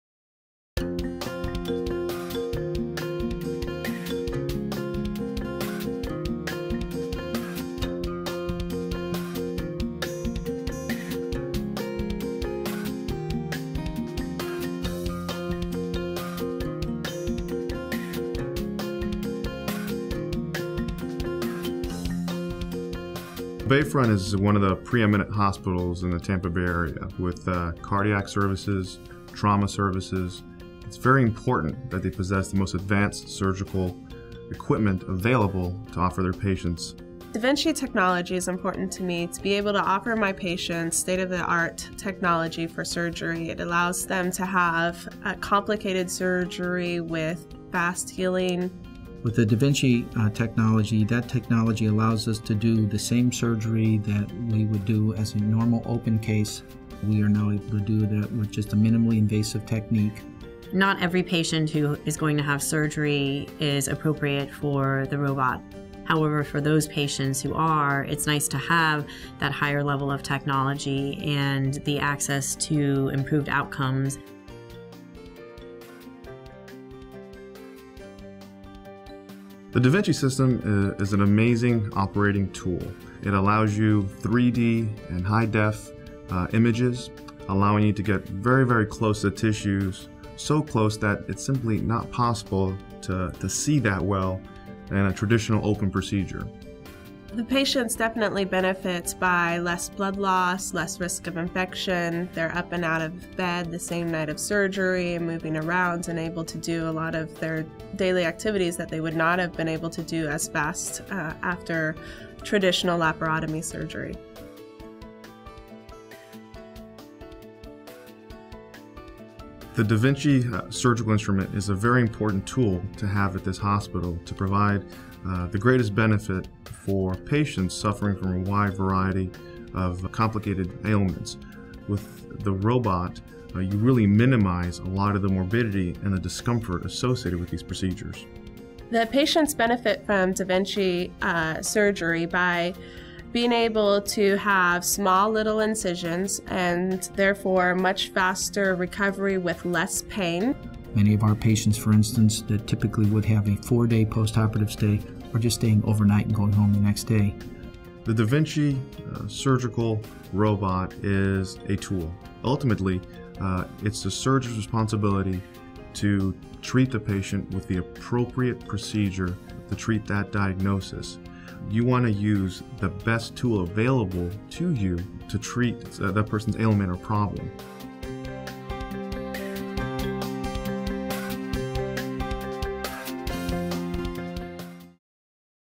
da Vinci Surgical System at Bayfront Medical Center with Dr. Christos Politis
The robotic arms of the Da Vinci Surgical System are controlled remotely by a surgeon. The system by Intuitive holds the promise of less invasive surgeries for prostate cancer patients and prostatectomy surgery.

St Pete Urology
830 Central Ave, Suite 100
St. Petersburg, FL 33701 USA